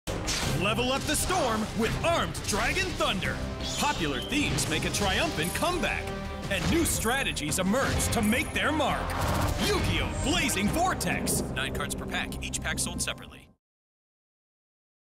Yu-Gi-Oh! TCG | Blazing Vortex | Available Now!
Get ready to Chazz it up in 2021 when Blazing Vortex hits the Yu-Gi-Oh! TRADING CARD GAME available everywhere now!

Level up your game with 100 new cards to play and collect including a reimagining of Chazz Princeton's "Armed Dragon" cards from the Yu-Gi-Oh! GX animated series. Play them on their own or combine them with the original "Armed Dragon" cards to create a hard-hitting new strategy that aims to unleash a brand-new version of Armed Dragon LV10 and power it all the way up to 10000 ATK!

Blazing Vortex both debuts a new combat-crazed World Premiere theme and spices things up with some World Premiere cards that turn the rules of engagement on their head, including a new Continuous Trap Card that forces Duelists to take turns playing their monster effects.